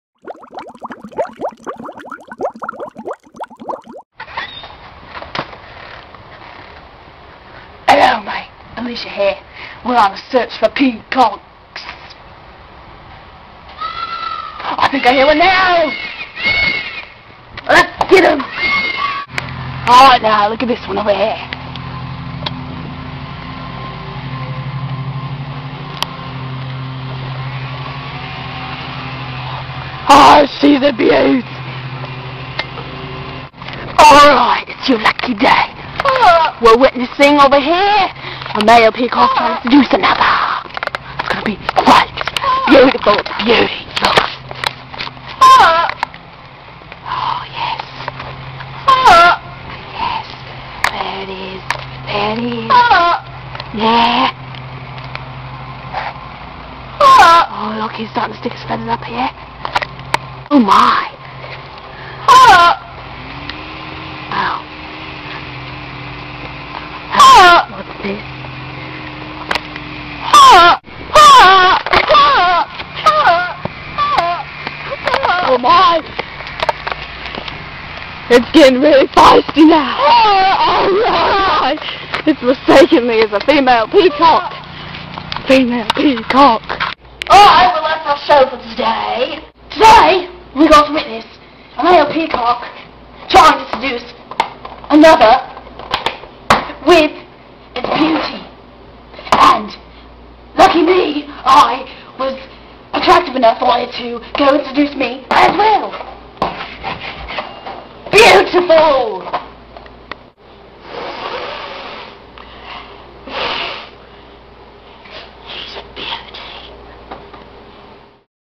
Peacock Safari.
It's expedition time! Animal under watch: Peacocks!